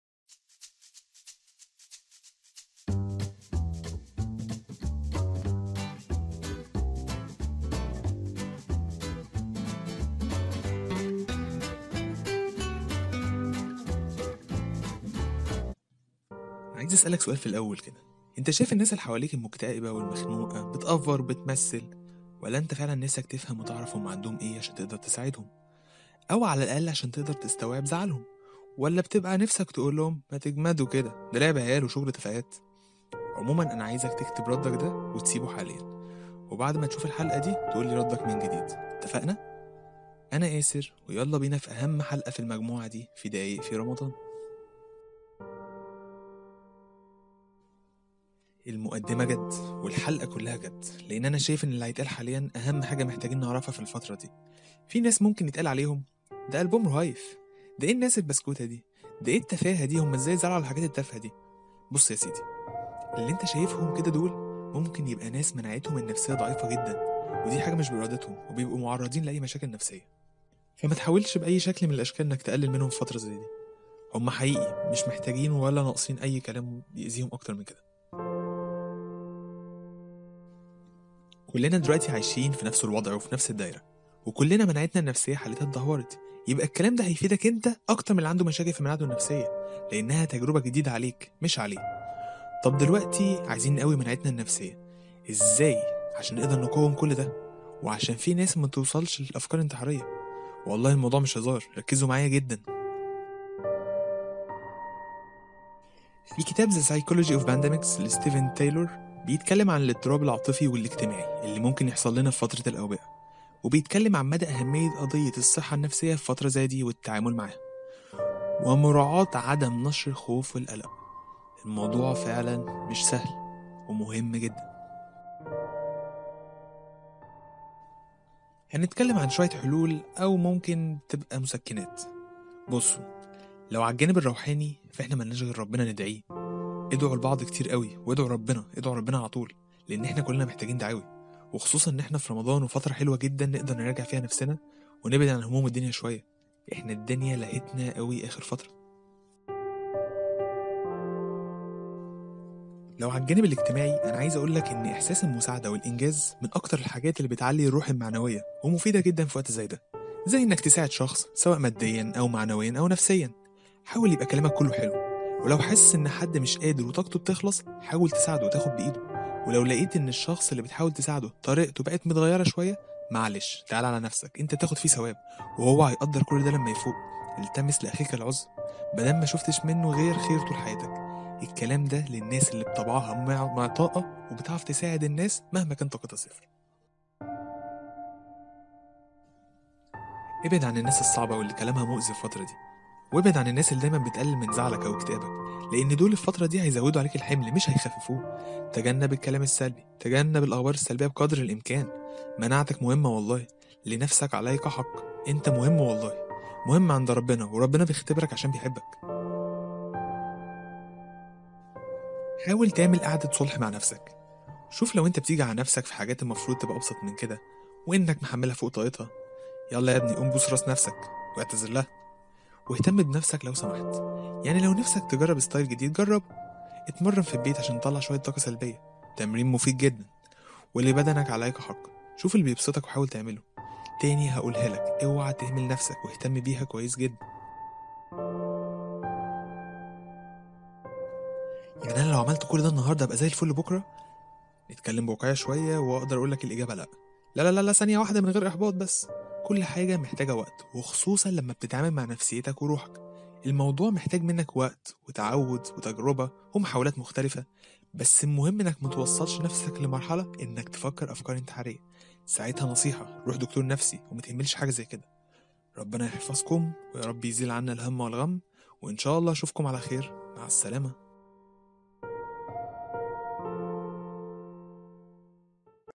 Minutes in Ramadan - EPS10 - Pandemics : Psychological Effects (2) الأوبئة : تأثيرات نفسية
Minutes in Ramadan

عدنا اليكم من جديد بجموعة حلقات جديدة في مجال مختلف ,بمناسبة ان احنا عايشين في زمن الكورونا ,علي مدار 5 حلقات الجايين هنتكلم عن الأوبئة برؤيا مختلفة وشكل جديد ,وهنشوف امثلة لاوبئة تانية ,وهنعرف تأثيرها علي كل حاجة حوالينا وهيبقي في تركيز في حلقتين عن التاثير النفسي لان دلوقتي باقينا كلنا ساكتين ونفسيا في مرحلة مش كويسة ,هنعرف الاسباب وهنقدر نوصل مع بعض لحلول مبدأئية.

انت لما بتشوف حد مخنوق او مكتئب حواليك ... ايه انطباعك عنه وبتحاول تساعده ولا لا ؟ طب لما انت نفسك تبقي في اكتئاب وضيق ايه اللي بتحاول تعمله ؟ ... طب احنا دلوقتي عرفنا اسباب ممكن تبقي هي اللي ممكن تبقي مخليه ده وضع اغلبنا النفسي حاليا ... يا تري في حلول ؟ عموما هنشوف مع بعض النهاردة حلول (وبتفرق من واحد لواحد جداااا ) ممكن نقدر نحاول فيها علشان نبقي افضل ونعرف نعدي الفترة دي علي خير باذن الله

حلقات علي مدار ايام رمضان متنوعة ,هيبقي فيه مواضيع مختلفة كتير ,سواء لاحداث تاريخية او اشخاص ذات تأثير او امراض او احداث غريبة ,ولكن كل موضوع هيكون ليه تأثير بشكل كبير علي فترة زمنية ما مر العصور

اتمني يكون في معلومات جديدة نقدر نستفاد منها بشكل دائم